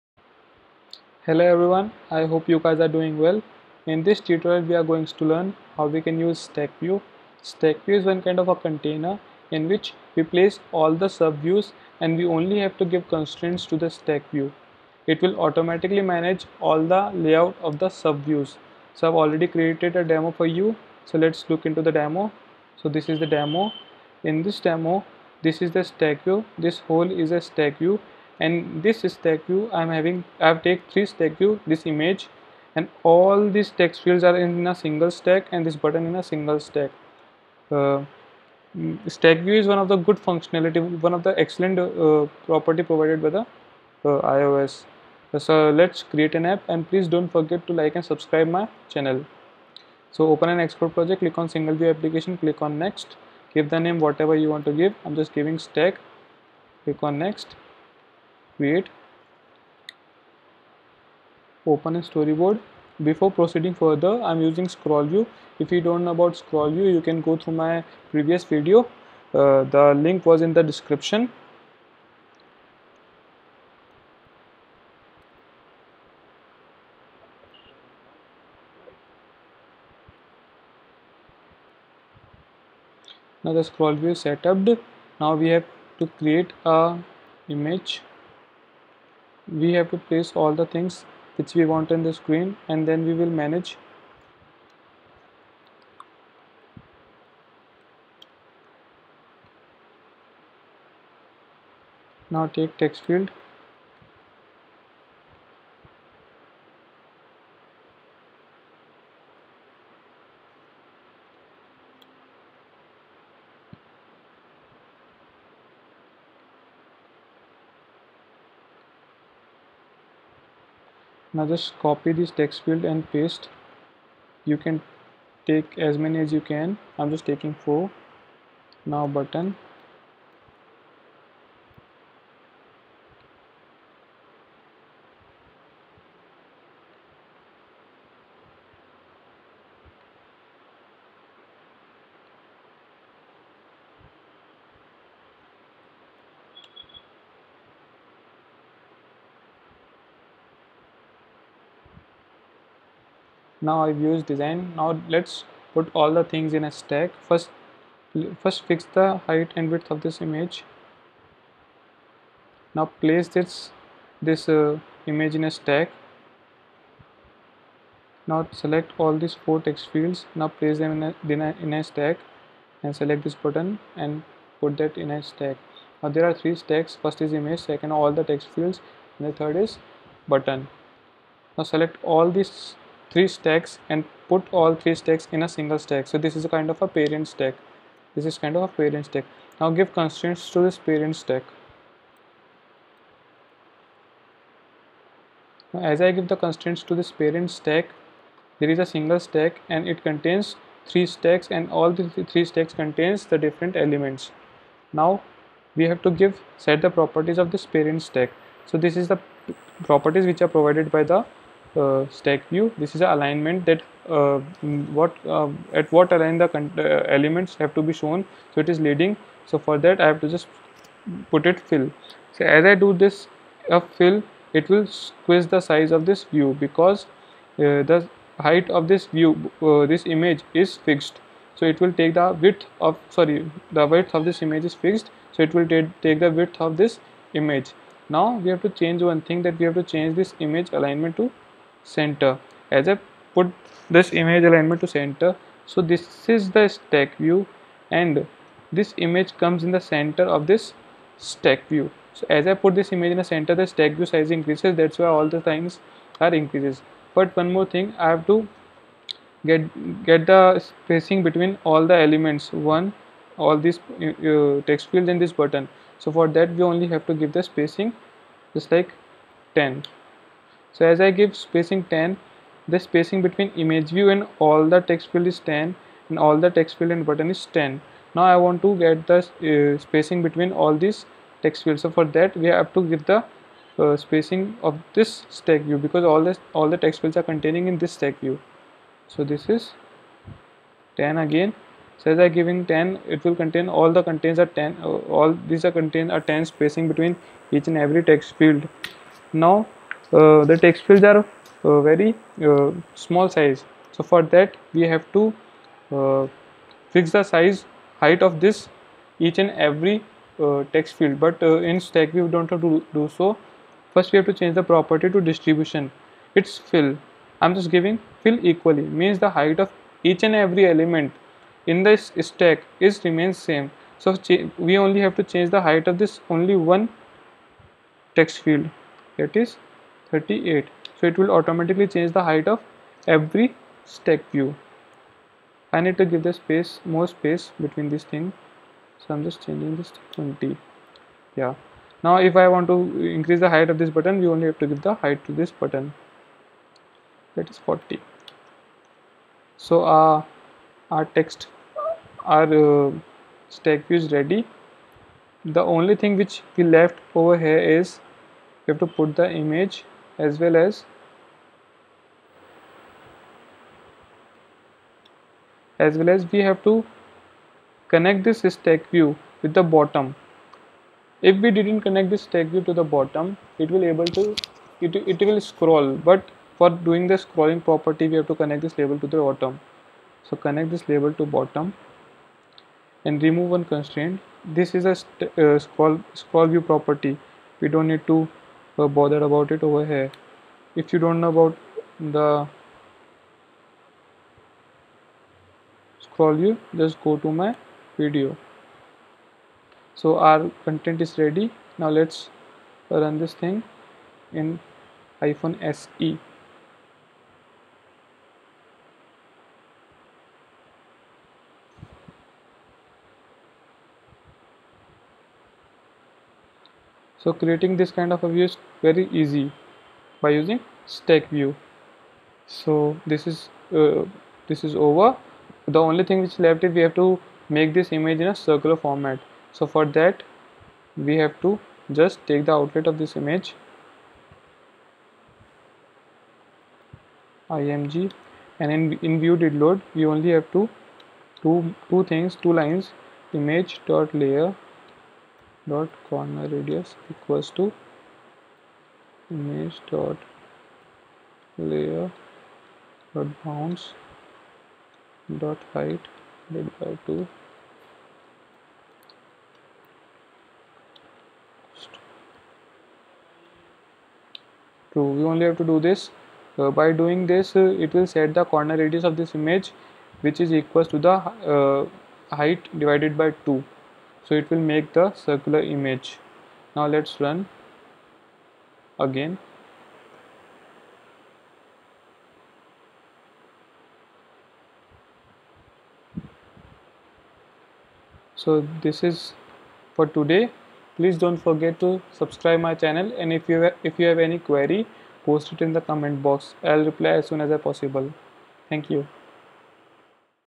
How to use StackView in Xcode 9.0 (Swift 4.0)
Hello Everyone,

In this tutorial we are going to learn about StackView. StackView is kind of a container which contain subviews and we only have to give the constraint to StackView and it will automatically manage the layout of subviews.

You can create the same UI without using stack views. But you will soon see how stack views completely change the way how you layout user interfaces.

You can embed a stack view in another stack view to build more complex user interfaces.

Don’t get me wrong. It doesn’t mean you do not need to deal with auto layout. You still need to define the layout constraints for the stack views. It just saves you time from creating constraints for every UI element and makes it super easy to add/remove views from the layout.

StackView Properties:
1. Axis :- Vertical / Horizontal
2. Alignment :- Help to align the arranged views.
3. Distribution :- Define size and position of arranged views.
4. Spacing :- Define the spacing between the subviews in stack.

Happy Coding.